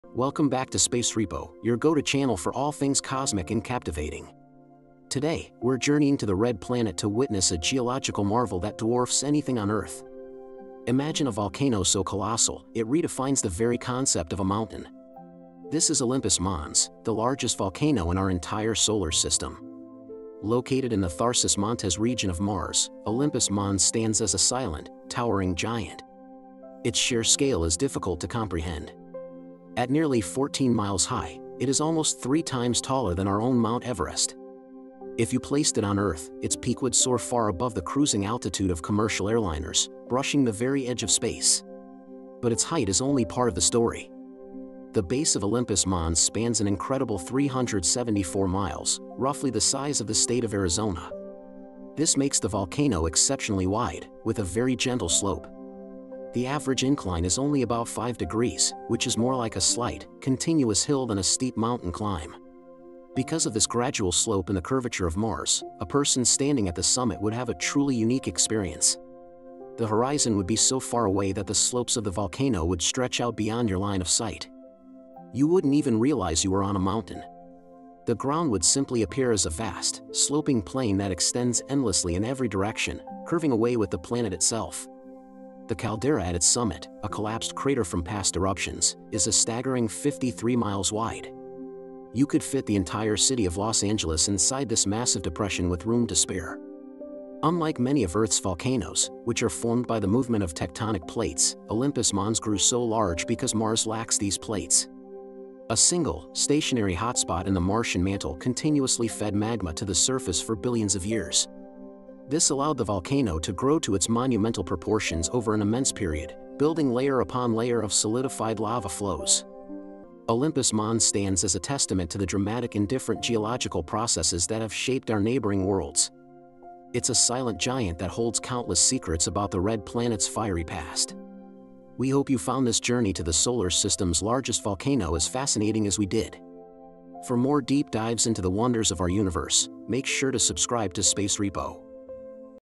Mars Has a Volcano 3x Taller Than Everest
Olympus Mons is the largest volcano in the solar system. It is so wide that if you stood on the peak, you wouldn't even know you were on a mountain. 🌋🏔️